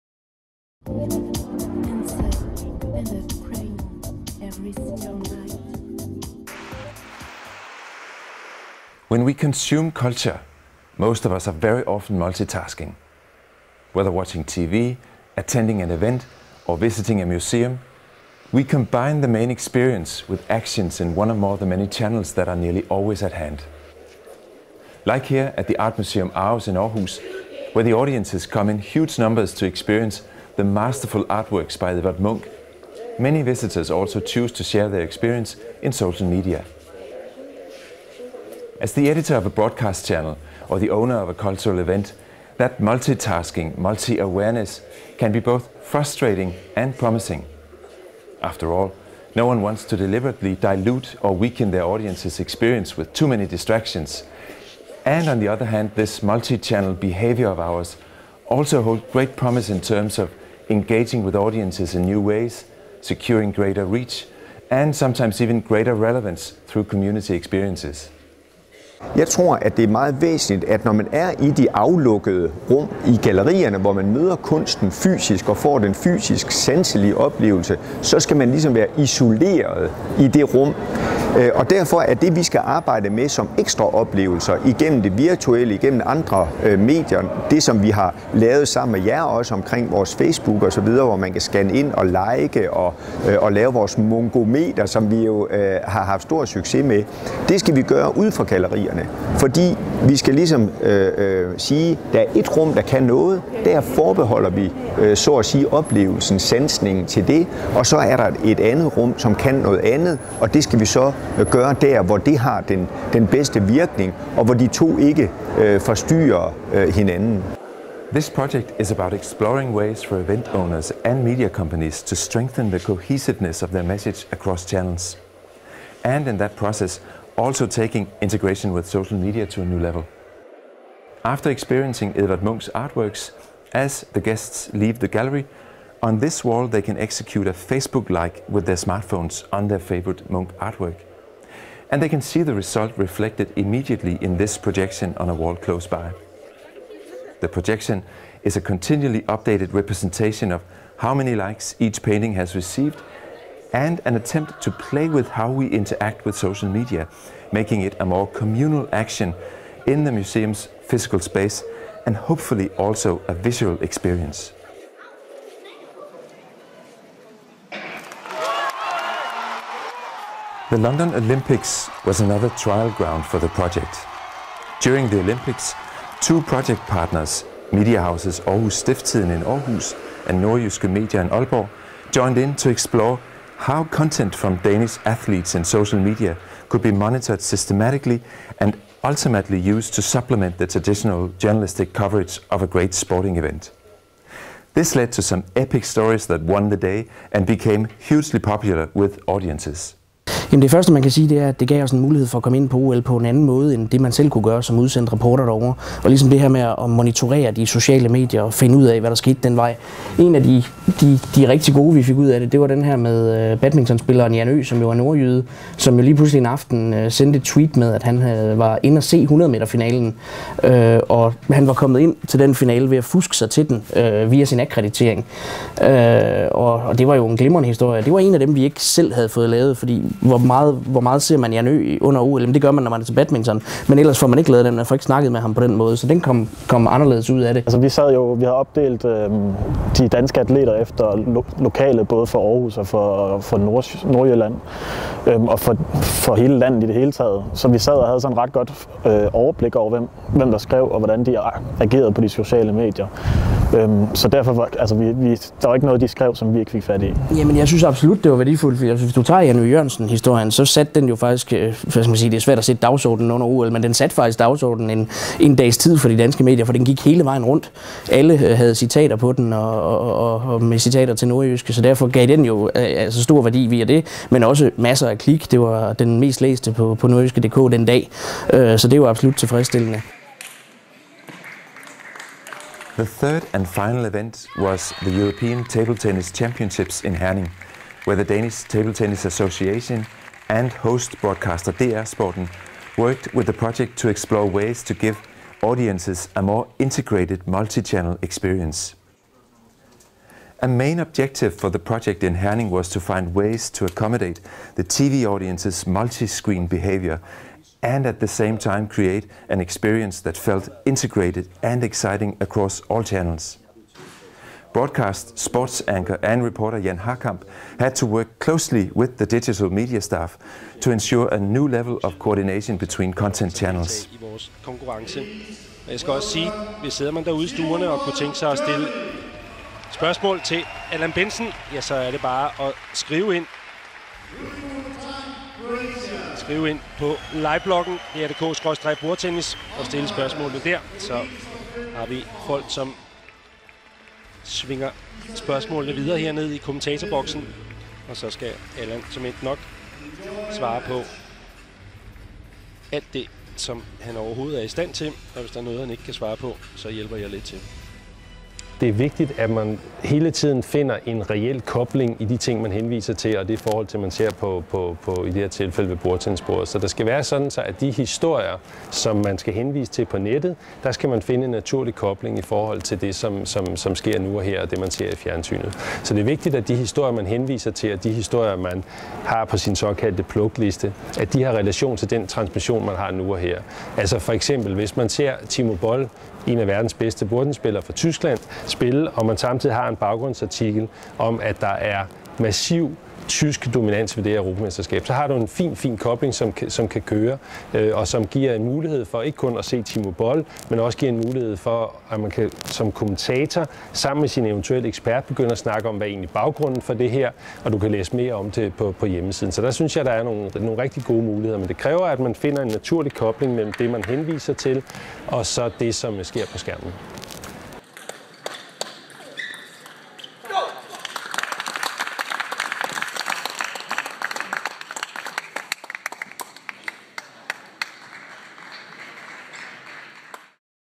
Shareplay & DSMJ: Eventbased Innovation (Integrated Media Experiences)
In the second half of 2012 Shareplay and the Danish School of Media and Journalism launched the project "Eventbased Innovation" with the aim of gaining new knowledge in the field of integrated media experiences. Watch the video to get an introduction to the project goals and activities. Camera, edit & voice over: Kristian Strøbech, @kstrobech.